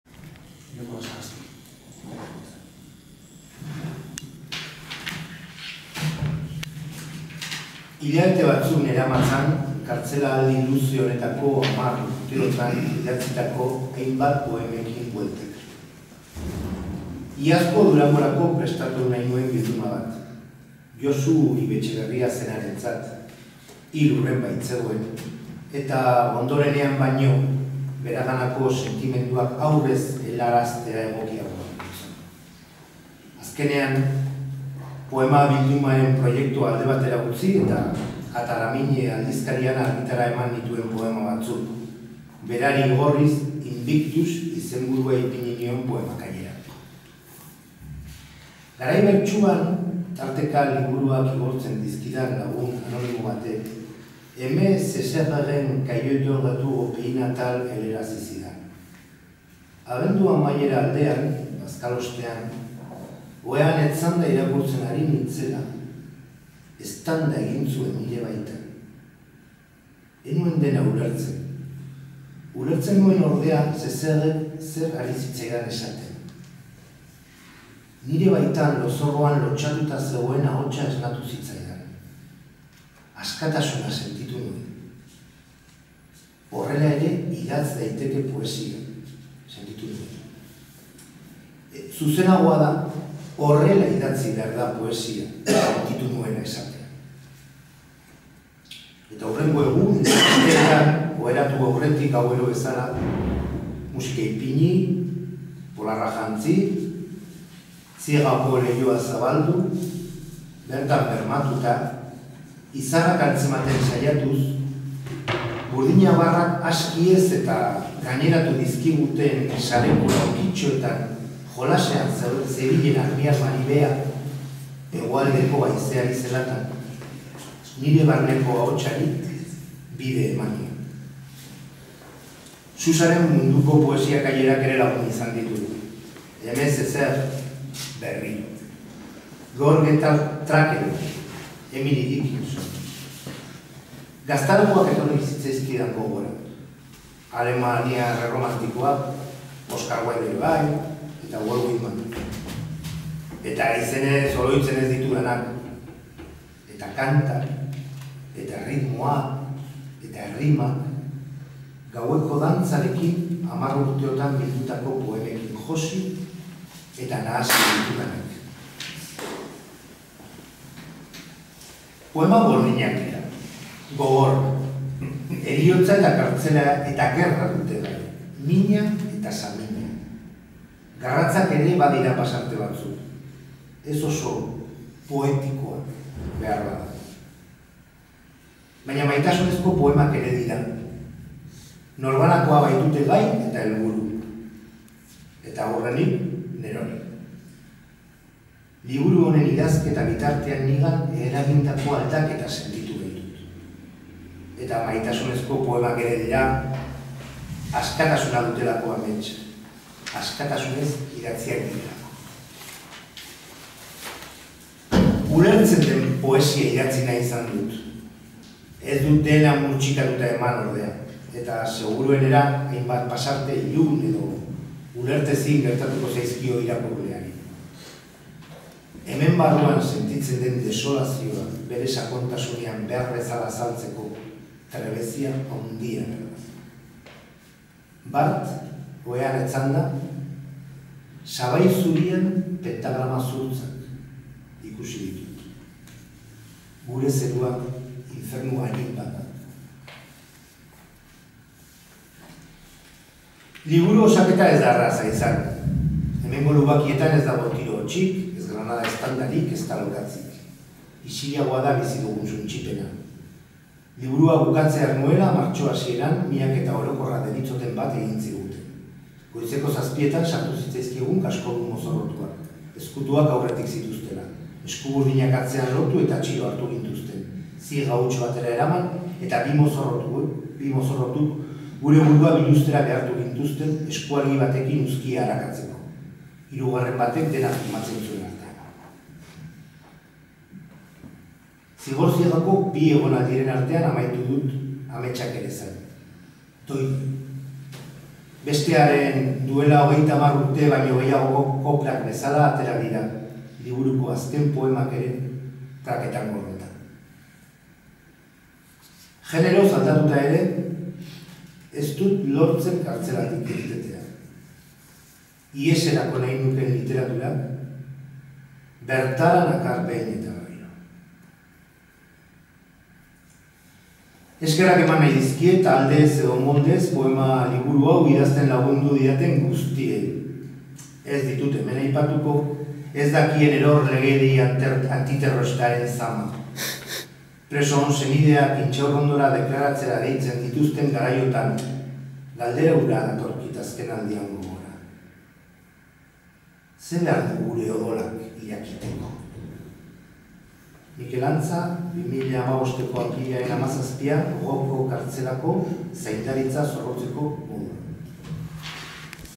Mikel Antzaren hitzak "Ametsak ere zain" poema-liburuz
Eneko Olasagasti zinemagile eta Antzaren adiskideak irakurri dut autoreak Réauko espetxetik bidalitako gutuna